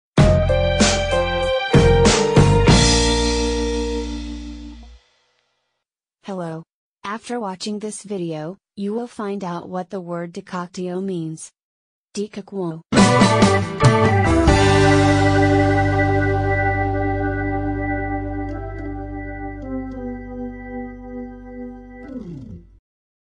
Definition of the word "Decoctio"
you will find full information about this word "decoctio".
From the video you will learn the definition for the word decoctio. The lexical meaning of the word decoctio. What does the word decoctio mean? And much, much more...